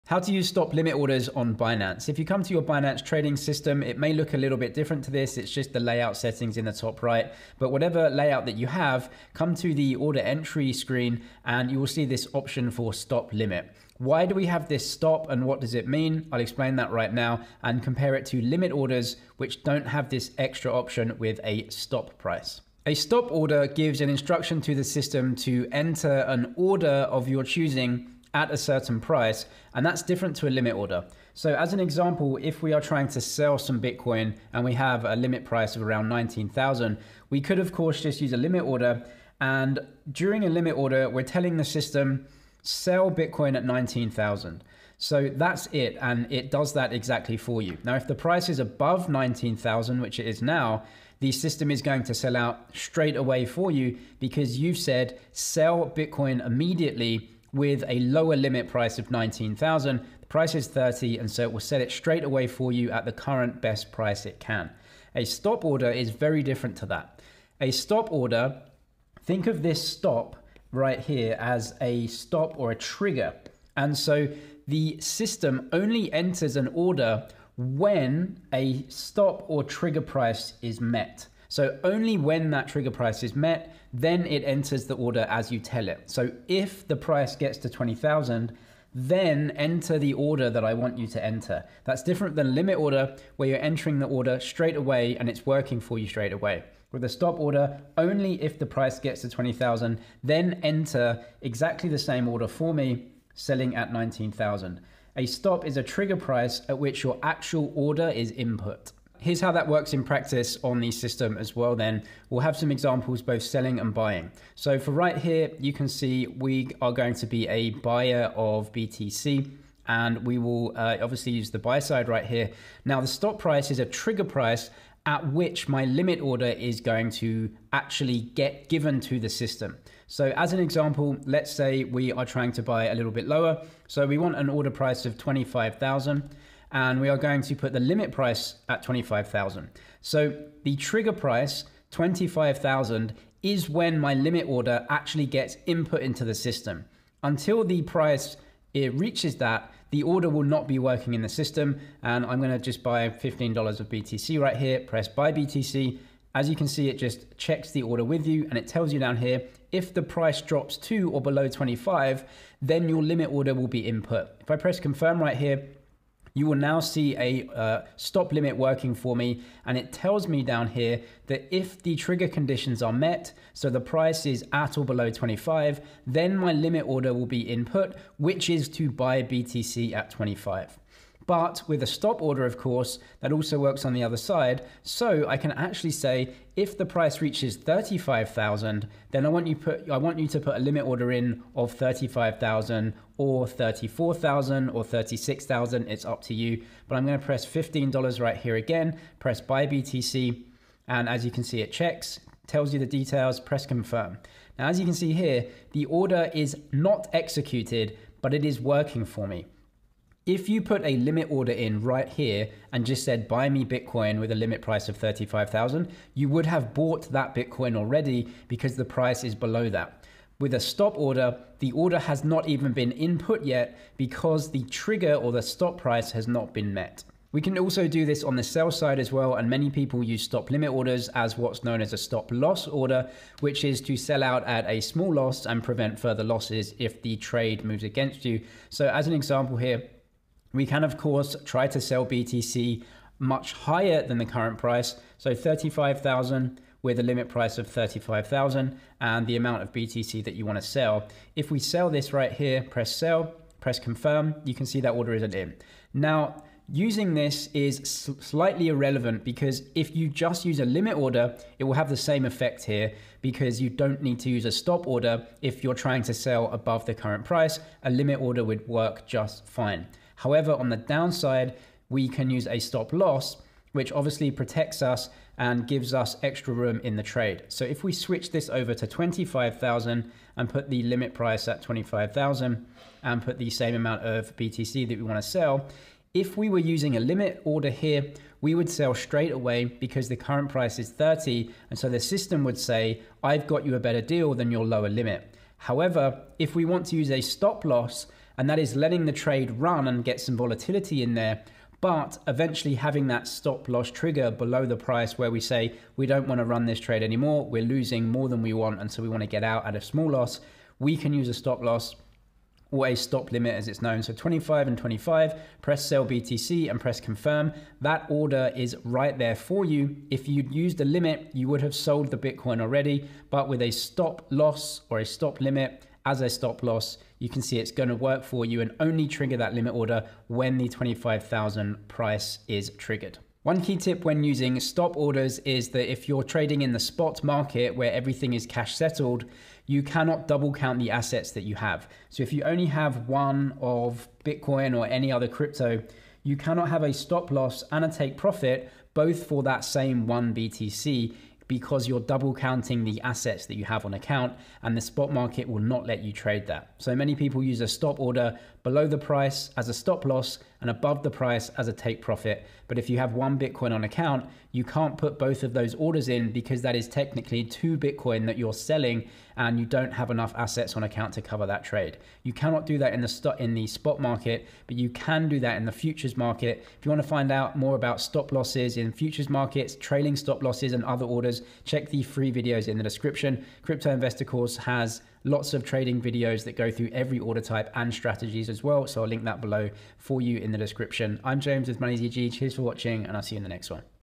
Binance Stop Limit Order Tutorial (Binance Stop Loss)
Binance stop limit order tutorial - what is Binance stop loss explained

TIMESTAMPS:
0:00 Binance stop limit order tutorial
0:22 Stop limit order explained
1:49 how to use stop Limit order (buy)
3:47 Binance stop loss order
5:33 Key tip